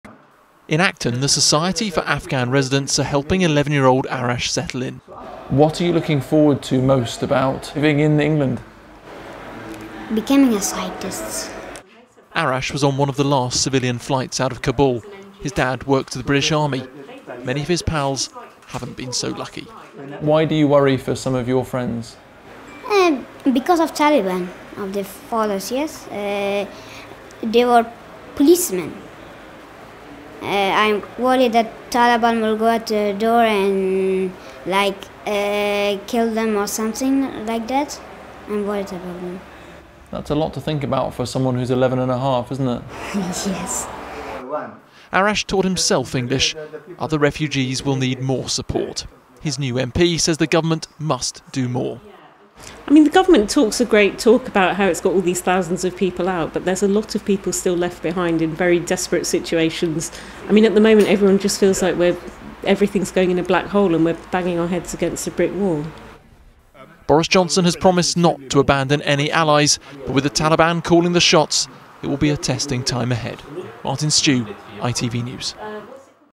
Afghanistan crisis: Rupa Huq MP welcomes 11-year-old refugee to West Acton
Video credit: ITV News

06/09/21